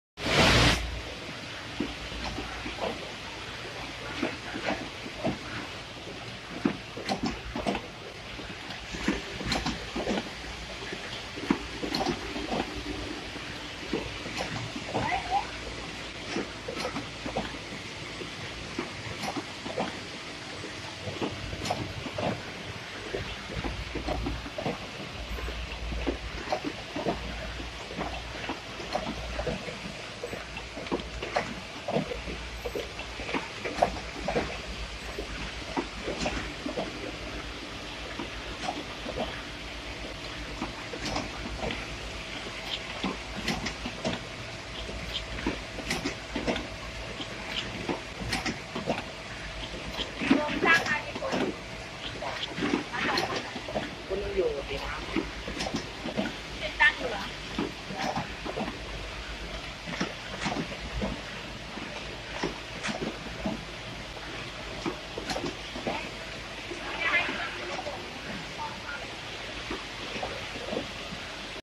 How to put the staples into carton box?
It is an automaitc machine to put the staples into the box.